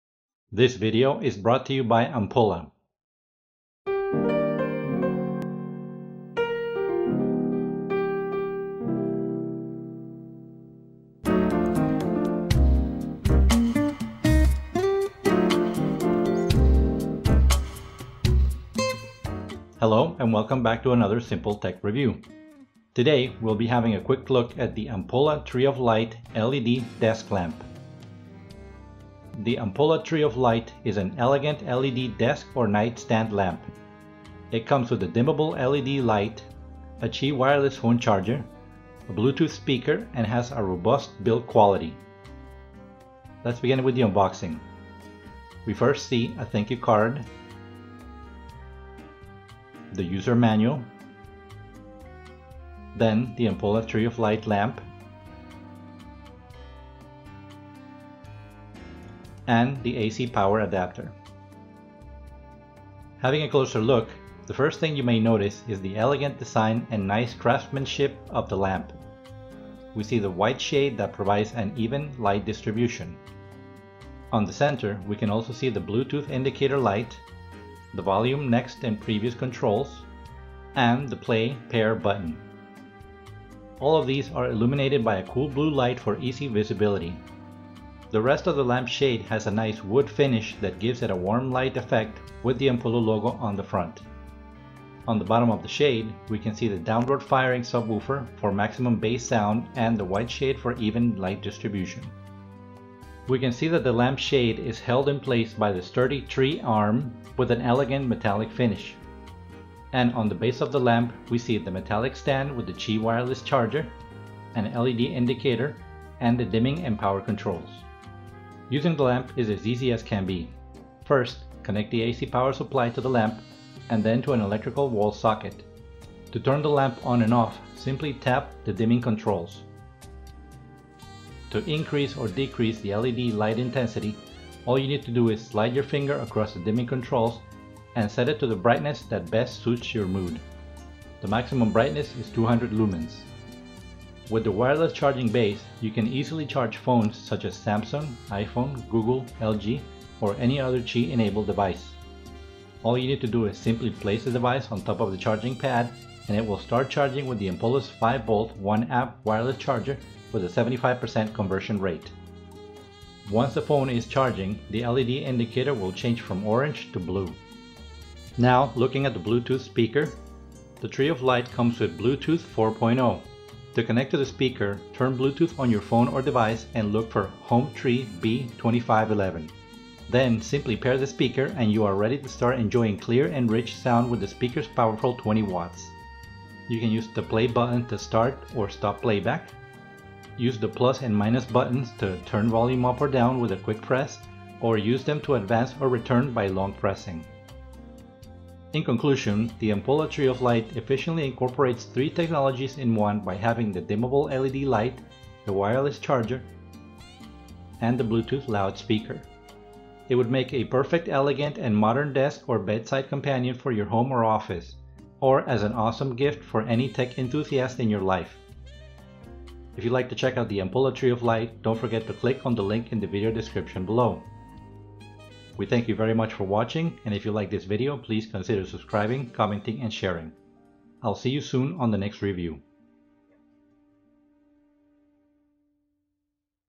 LED Lamp, Wireless Charger & Bluetooth Speaker - Ampulla Tree of Light
Elegant, Modern and Sturdy!
The Tree of Light comes with a Dimmable LED light, a Qi Wireless charger and a Bluetooth 4.0 speaker with rich sound! It's a perfect bedside or office lamp!